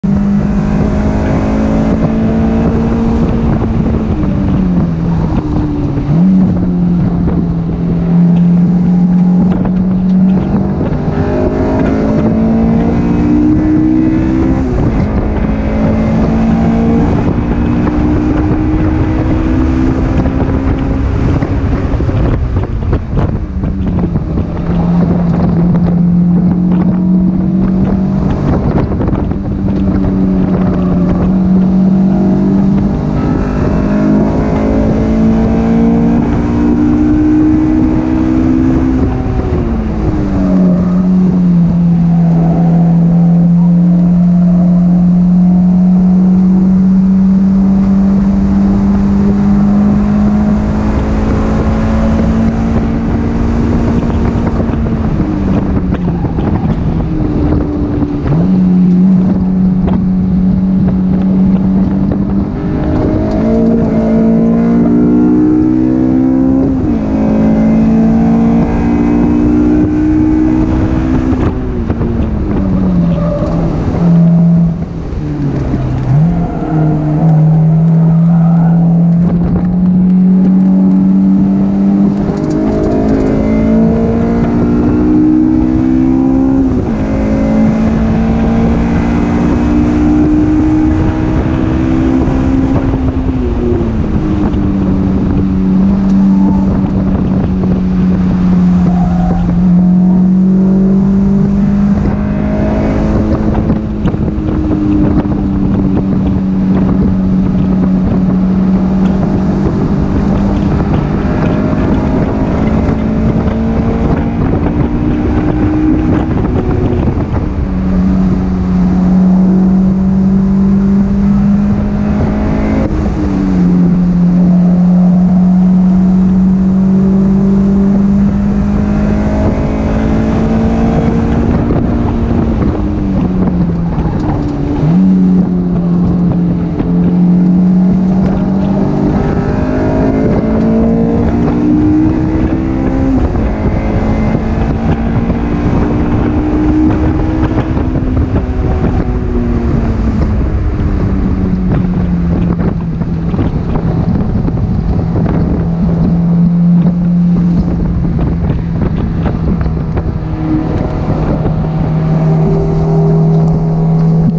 Chuckwalla Valley Raceway 11/21/20
Here is a lap of my best time of the day: 2:07.0

2004 Honda S2000
Tractive Suspension
Hardrace suspension bushings, ball joints, RCAs, Front sway bar
AP1 steering rack and flywheel
Hawk DTC 70 pads, stock calipers and rotors
949 Racing 6UL 17x9 square
Maaxis RC1 255 square
No aero and top down!